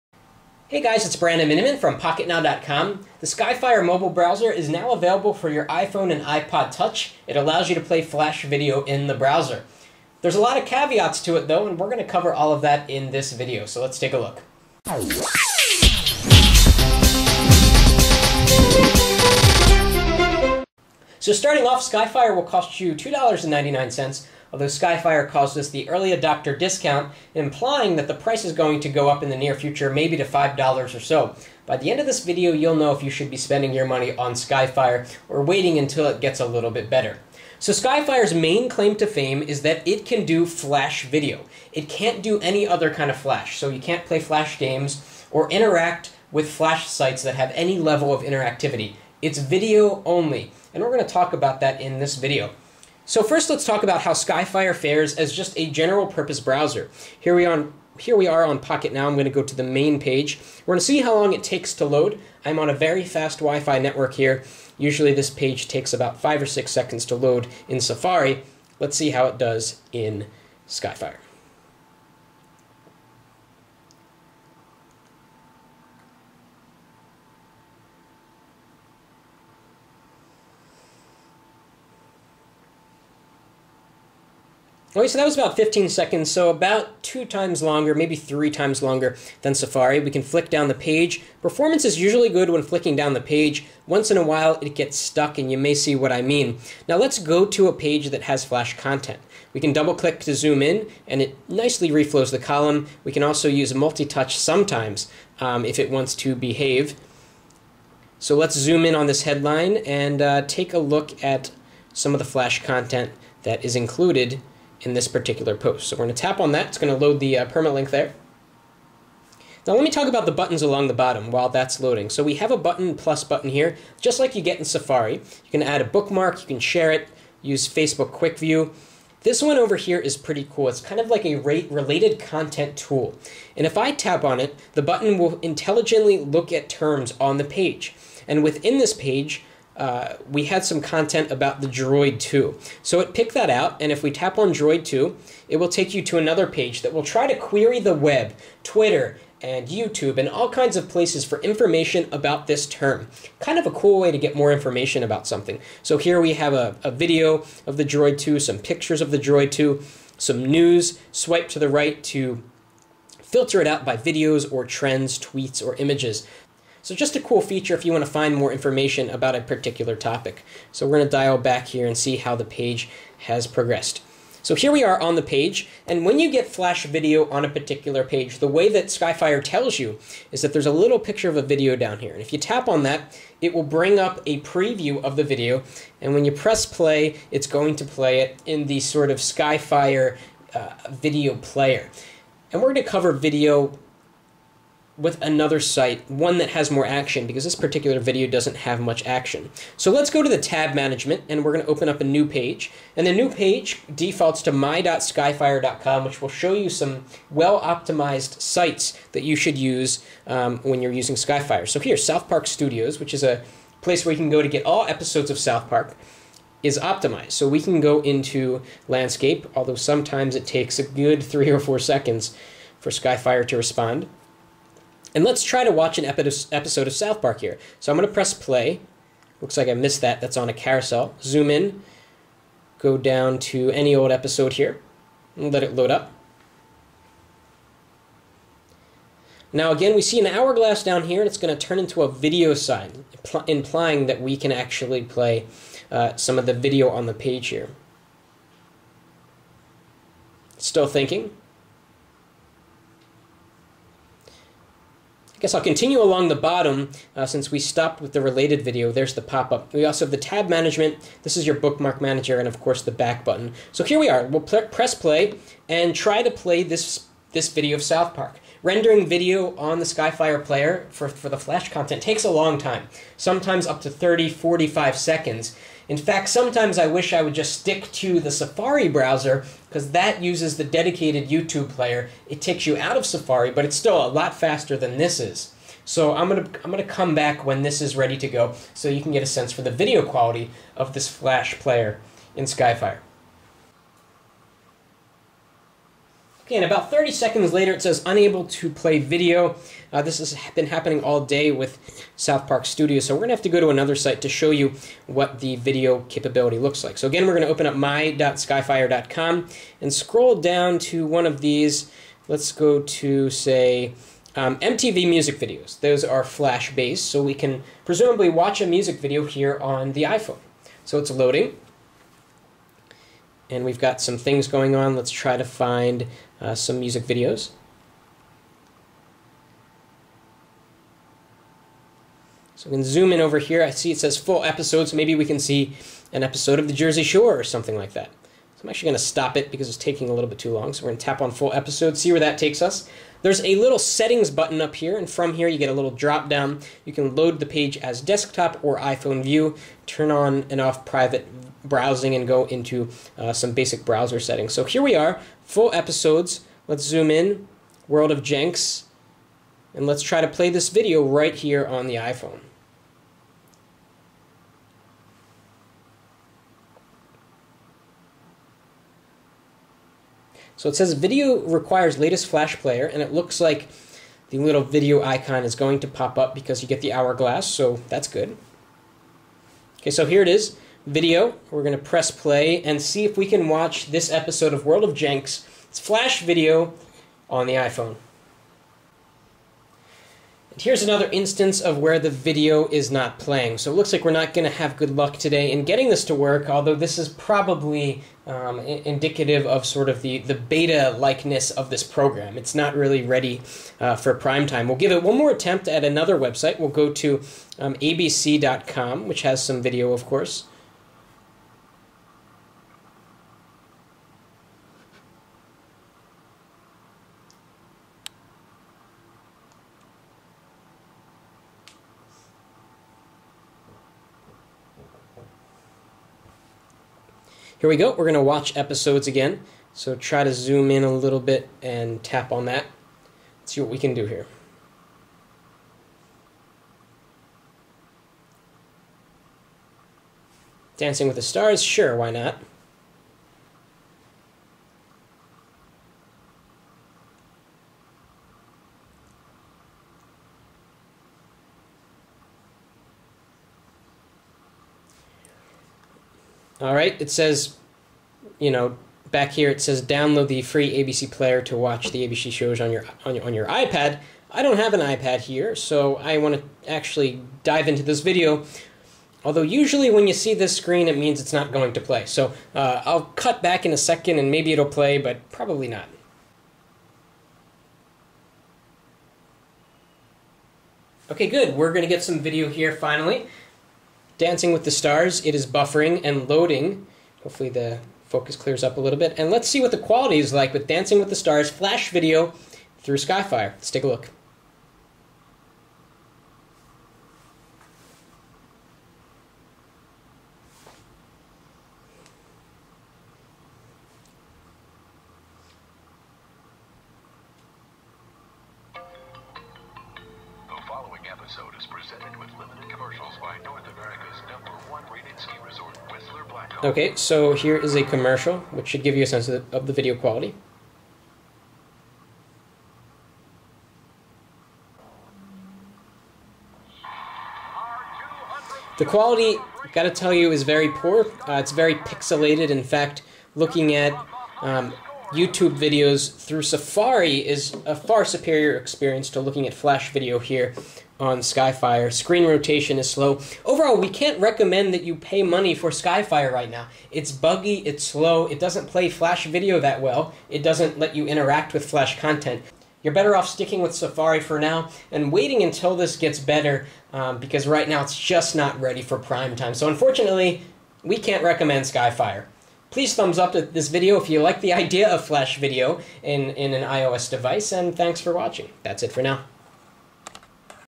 Skyfire for iPhone and iPod Touch Does Flash | Pocketnow
Here's a look at how Skyfire works with Flash video found on the web.